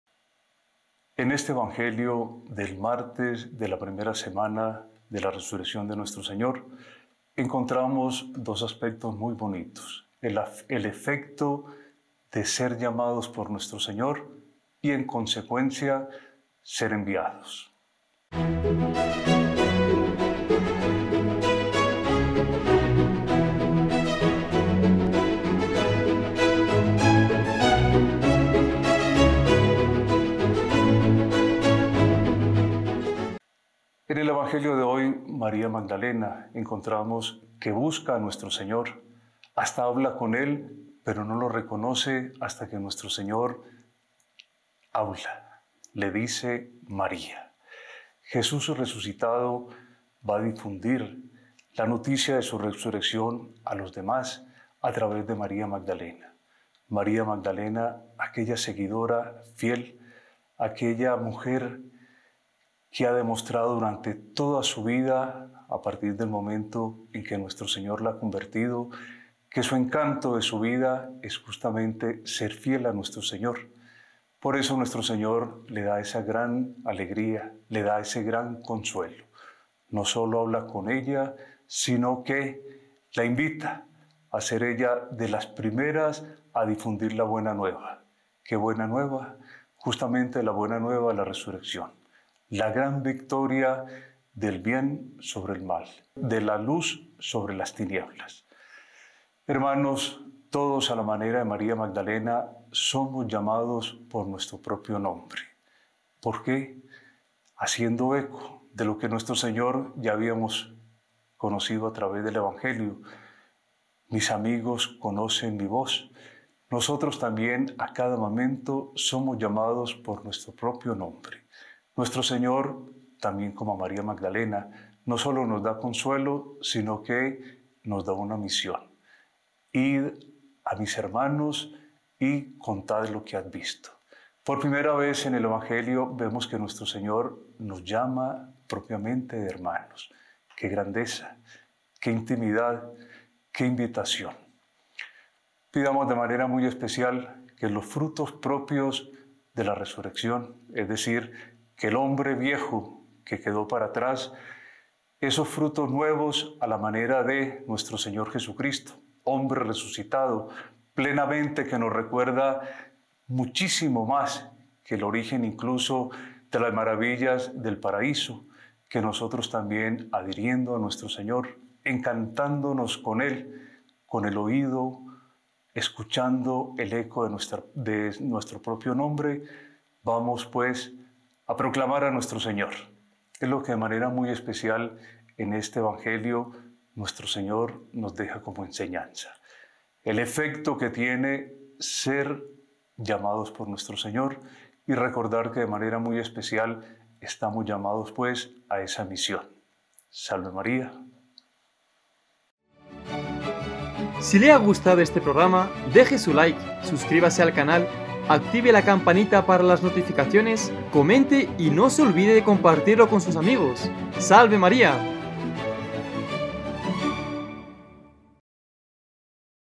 Evangelio de hoy - Martes 19 de Abril - Dios nos llama a todos
EvangeliodeHoy con el Padre Álvaro Mejía, EP

El día de la resurrección, María se había quedado llorando junto al sepulcro de Jesús. Sin dejar de llorar, se asomó al sepulcro y vio dos ángeles vestidos de blanco, sentados en el lugar donde había estado el cuerpo de Jesús, uno en la cabecera y el otro junto a los pies. Los ángeles le preguntaron: “¿Por qué estás llorando, mujer?” Ella les contestó: “Porque se han llevado a mi Señor y no sé dónde lo habrán puesto”.

Dicho esto, miró hacia atrás y vio a Jesús de pie, pero no sabía que era Jesús. Entonces él le dijo: “Mujer, ¿por qué estás llorando? ¿A quién buscas?” Ella, creyendo que era el jardinero, le respondió: “Señor, si tú te lo llevaste, dime dónde lo has puesto”. Jesús le dijo: “¡María!” Ella se volvió y exclamó: “¡Rabuní!”, que en hebreo significa ‘maestro’. Jesús le dijo: “Déjame ya, porque todavía no he subido al Padre. Ve a decir a mis hermanos: ‘Subo a mi Padre y su Padre, a mi Dios y su Dios’ ”.

María Magdalena se fue a ver a los discípulos para decirles que había visto al Señor y para darles su mensaje.